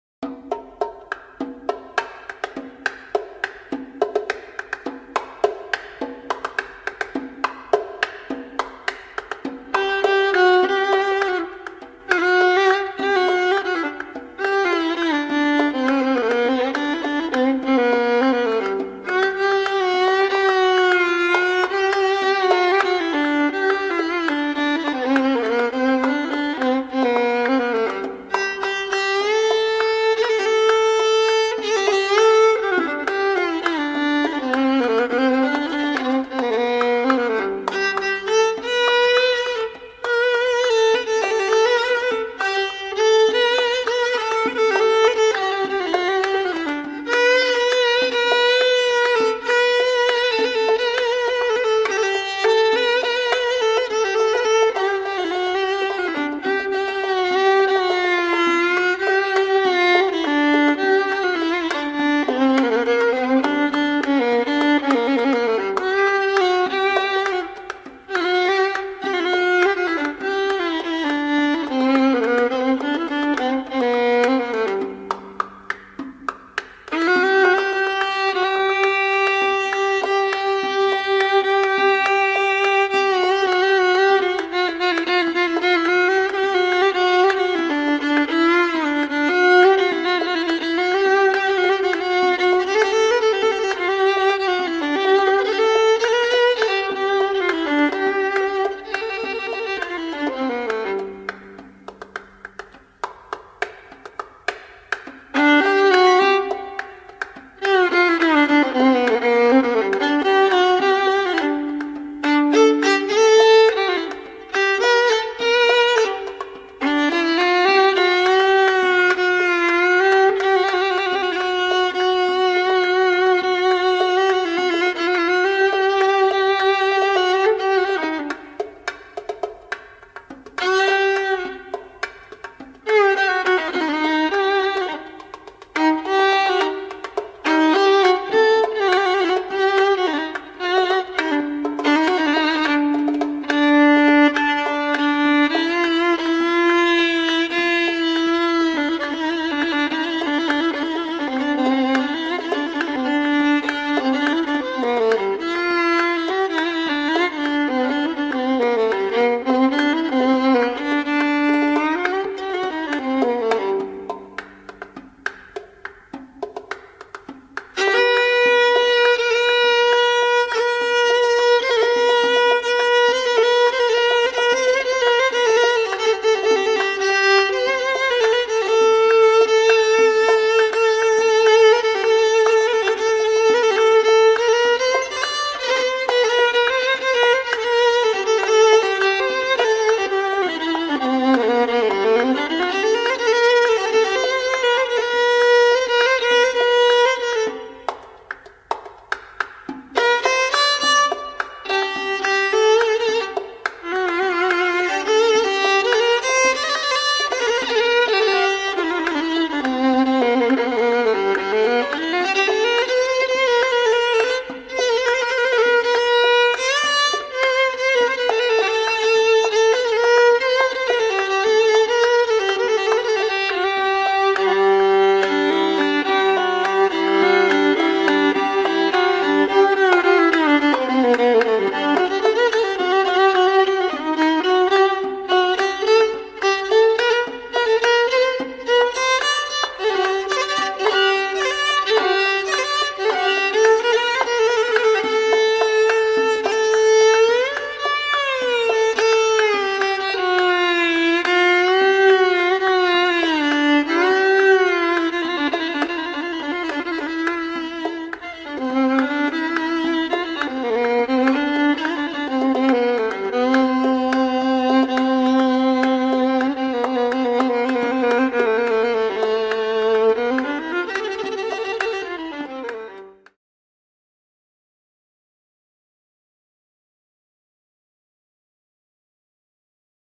Violin Taqasim | Daoud Akram (violin) Yaakob Mourad (bongos) | jewish Iraqi Musicians
תקסים כינור מהנגן הגדול דאוד אכרם בלווי נגינת הבונגוס של יעקב מראד האגדי זכרם לברכה .
ערוץ המוסיקה המזרחית קלאסית של המוסיקאי אריאל כהן